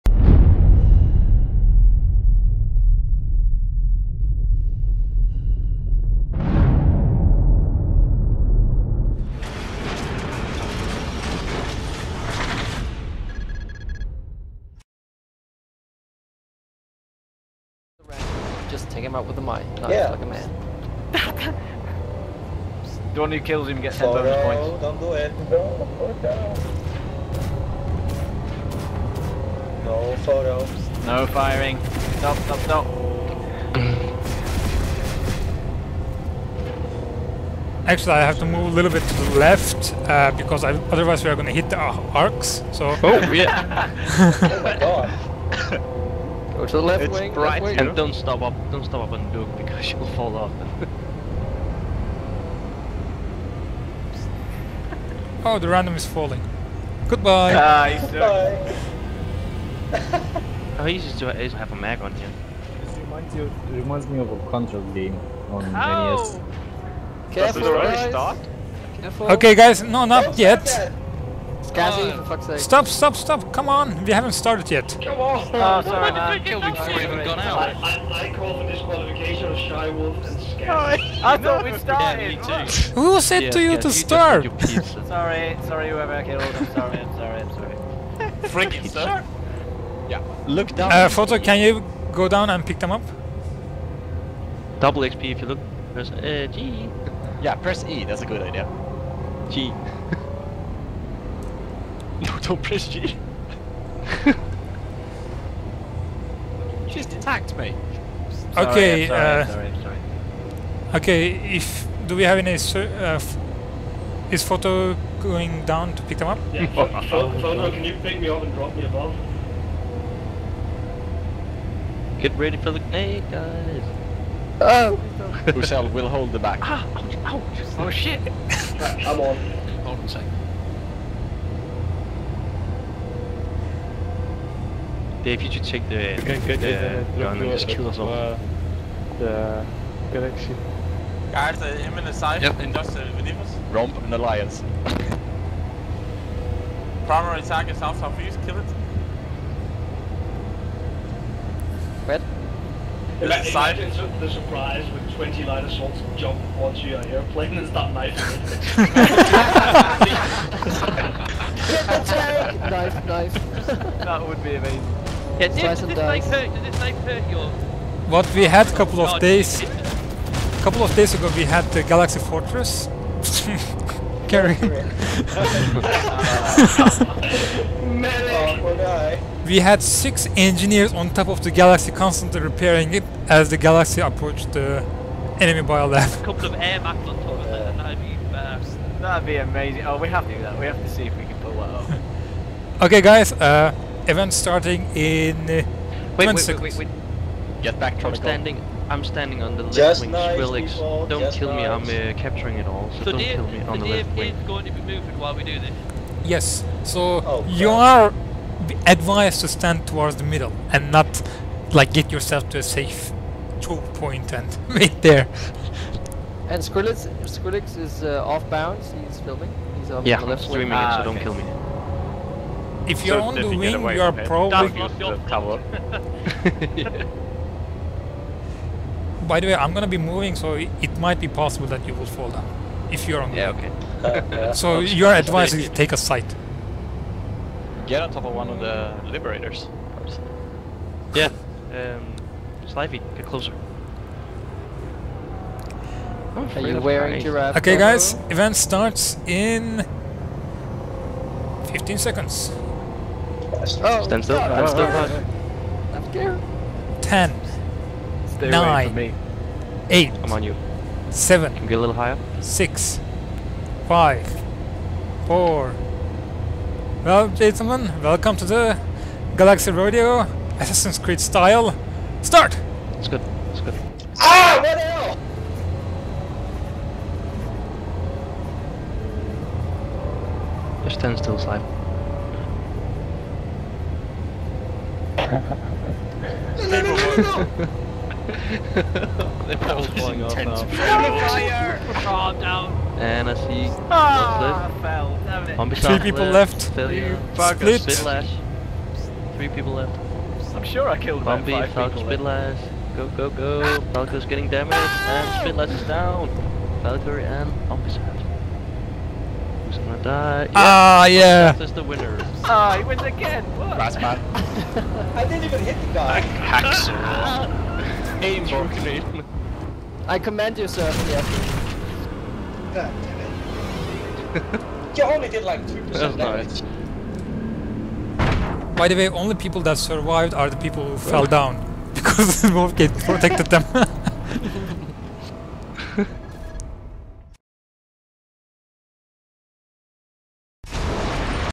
Casual Friday Night Events - DWG - Casual Friday Night Event - Galaxy Assassin's Creed of 14.12.2012 (3 of 4)
Weekly event organized by Deathwatch Gaming outfit in Miller server.